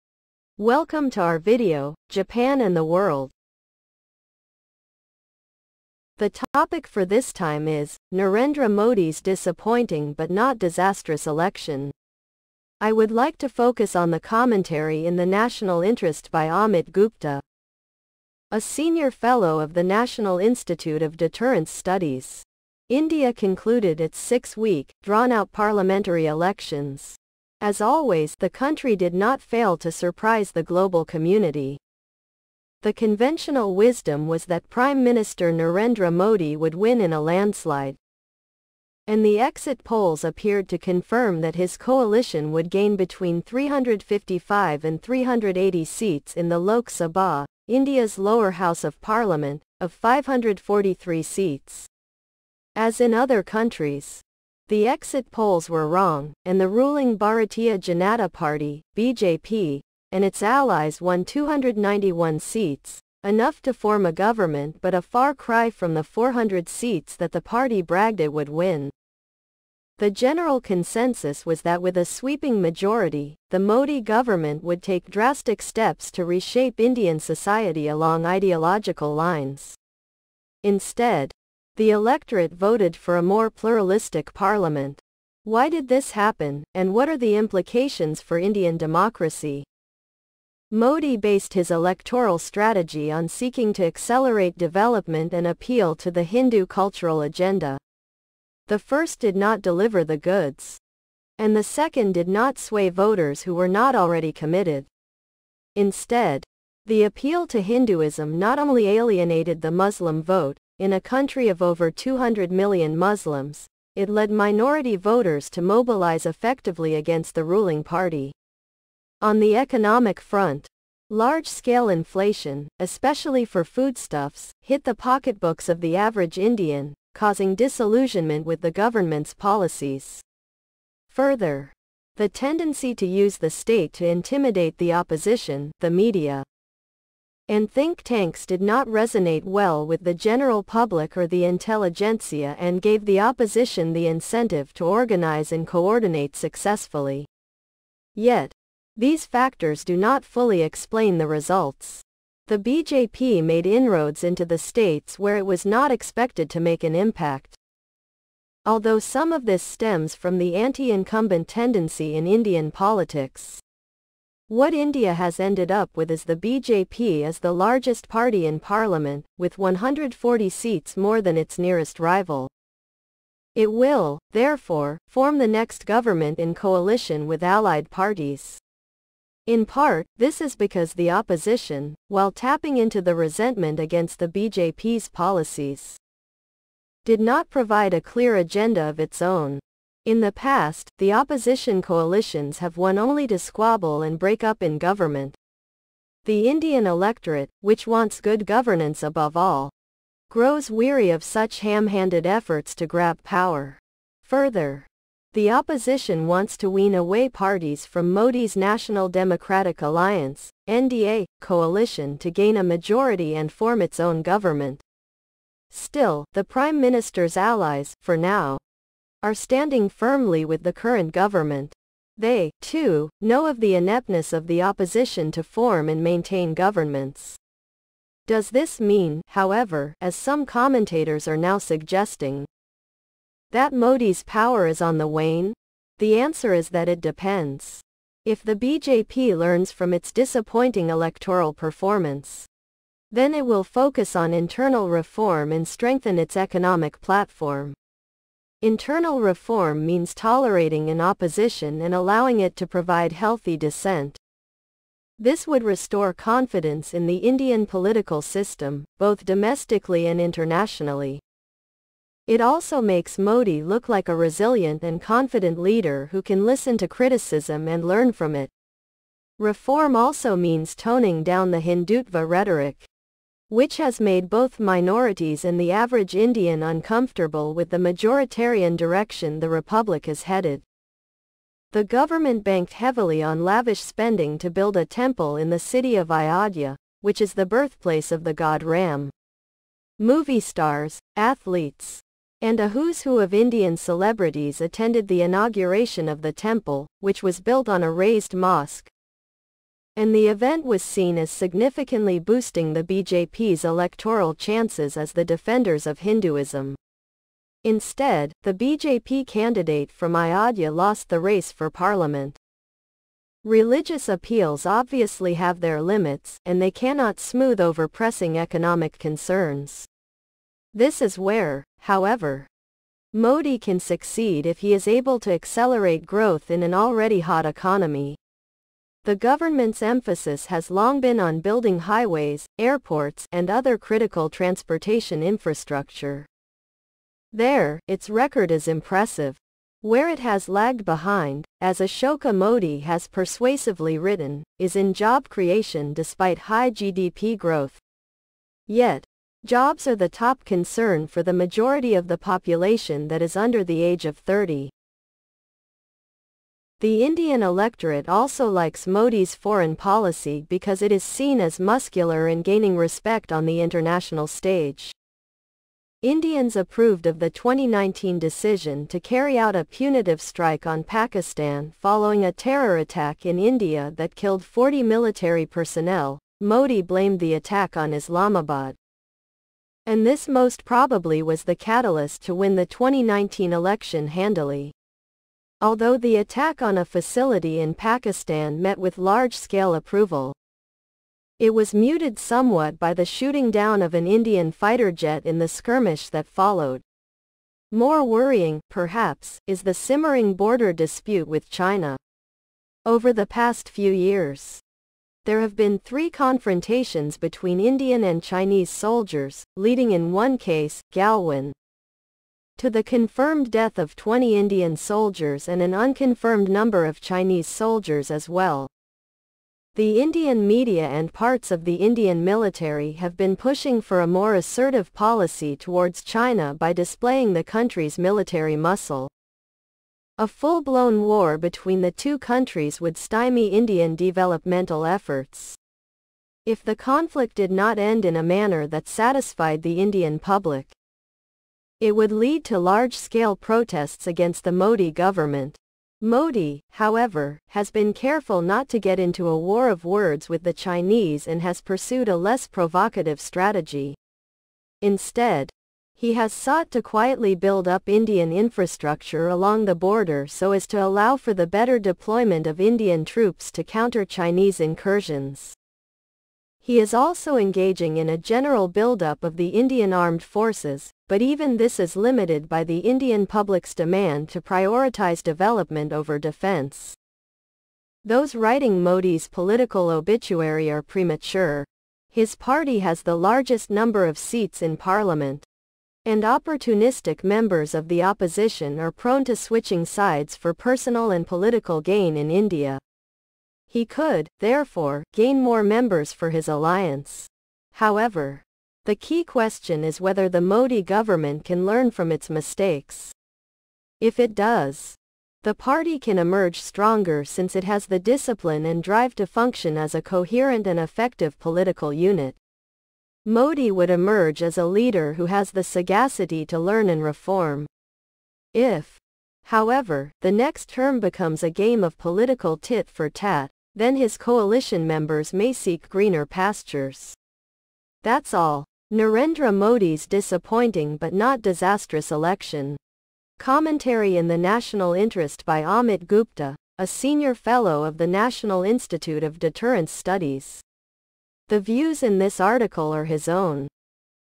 Narendra Modi’s Disappointing But Not Disastrous Election.
by Amit Gupta, a Senior Fellow of the National Institute of Deterrence Studies. The views in this article are his own.